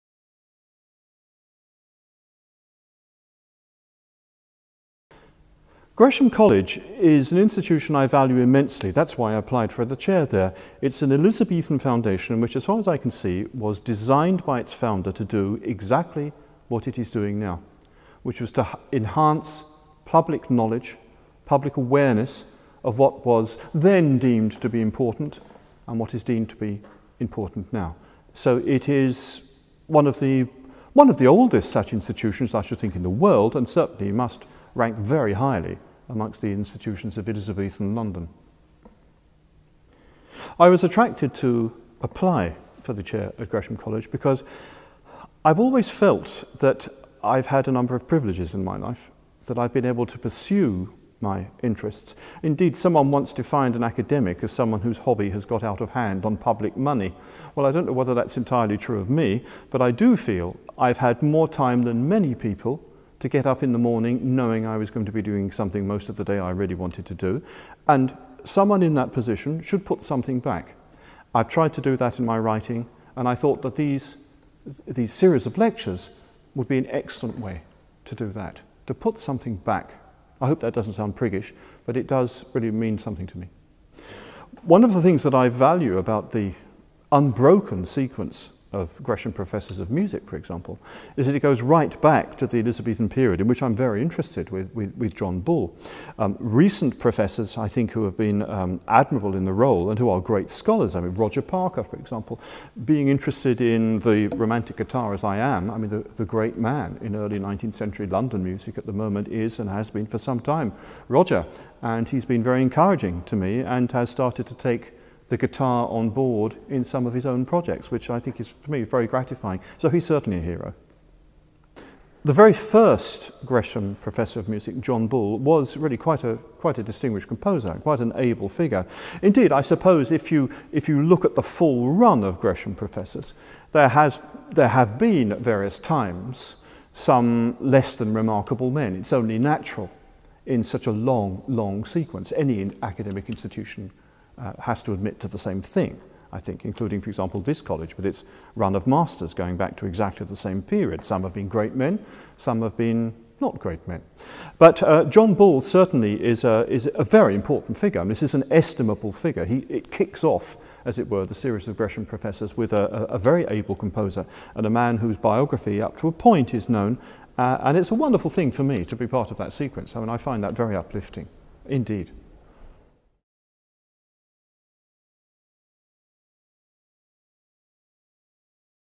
The Value of Gresham College - Professor Christopher Page
Christopher Page discusses what his appointment as the latest Gresham Professor Means to him. His Series of Lectures can be found

Professor Page talks about how it feels to be Gresham Professor of Music, and highlights the achievements of his predecessor. He discusses the first Gresham Professor of Music John Bull, as well as highlights his personal debt to a more recent professor, Roger Parker.

Christopher Page is the Gresham Professor of Music. Information on his ongoing series of free public lectures is available